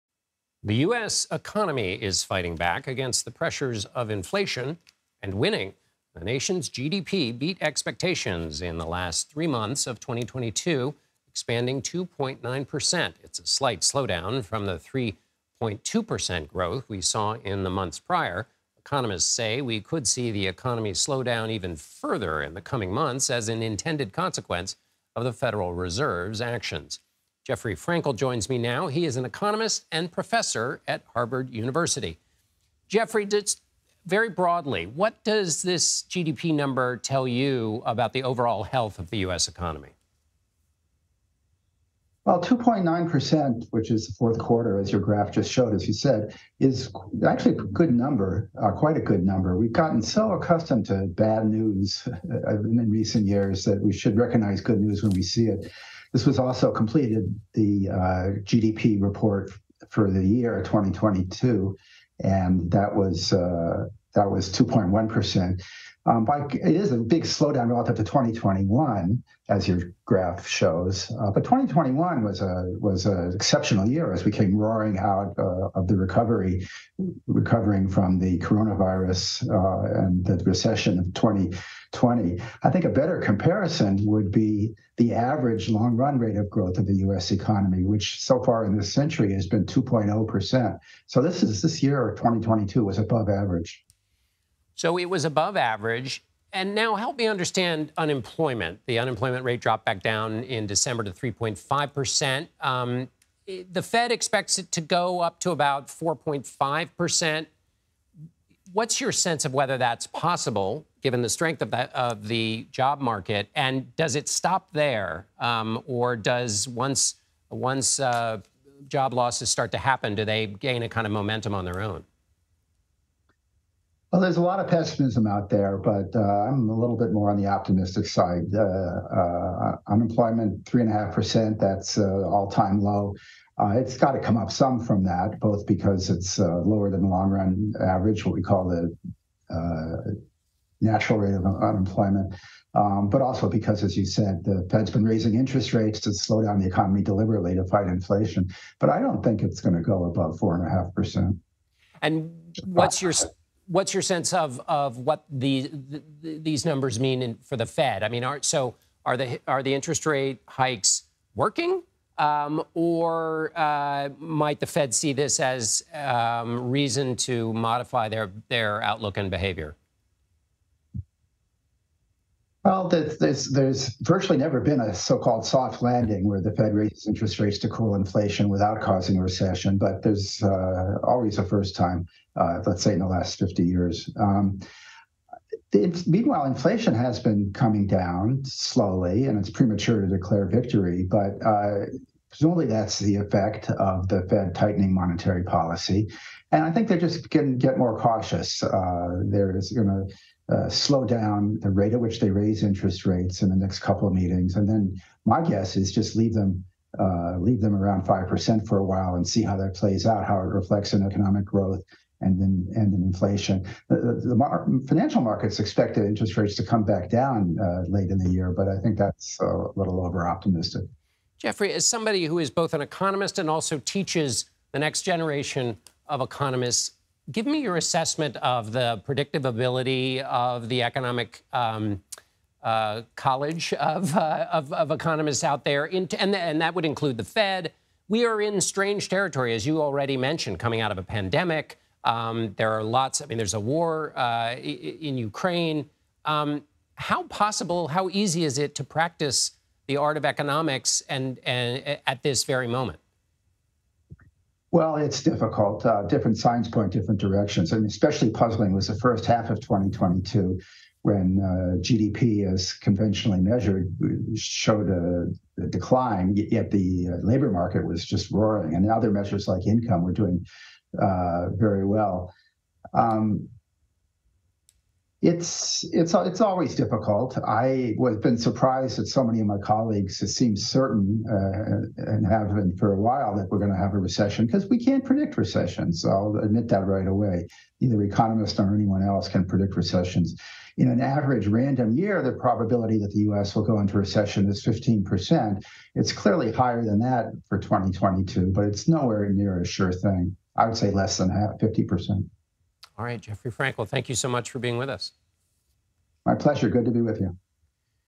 U.S. economy grew to end 2022, unemployment rate stays low
The economy is fighting back against inflationary pressures, but many economists say that won't last, and the economy could slow down in 2023 as an intended consequence of the Federal Reserve's efforts to rein in inflation by raising interest rates. Economist and Harvard University professor Jeffrey Frankel joined CBS News to discuss what the latest GDP data could mean for the economy moving forward.